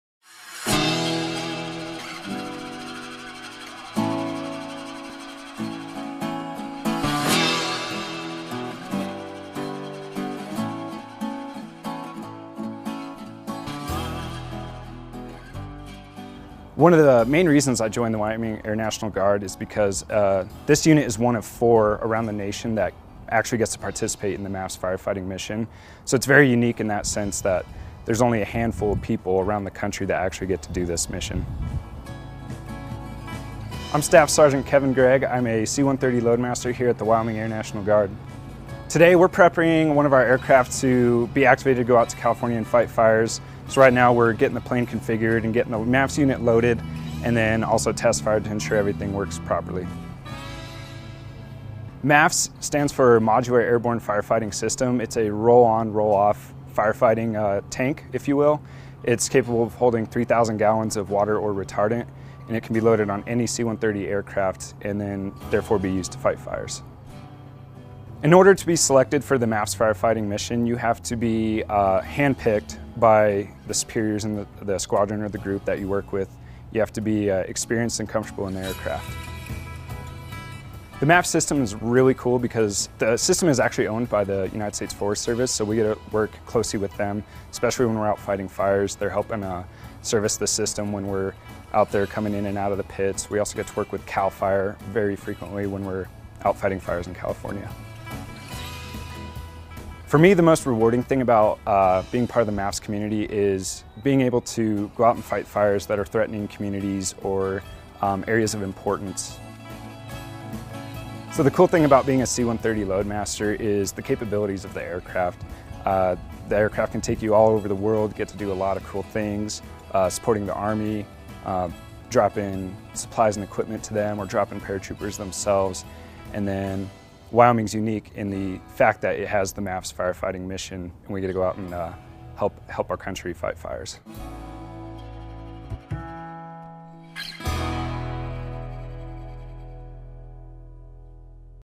Wyoming National Guard MAFFS activated for for western wildfires
U.S. Air Force Staff Sgt. Kevin Gregg, a loadmaster with the 187th Airlift Squadron, shares what it is like being part of a MAFFS (Modular Airborne Fire Fighting System) activated C-130 Hercules aircraft.

On July 19, 2021, members assigned to the 153rd Airlift Wing prepare and load the C-130 Hercules aircraft with a U.S. Forest Service MAFFS in Cheyenne, Wyo. The C-130H aircraft departed to California on July 20, 2021. The Wyoming Air National Guard MAFFS unit is activated to support the western area wildfires, and the unit will operate out of McClellan Park, Calif. (Video by Jacqueline Marshall)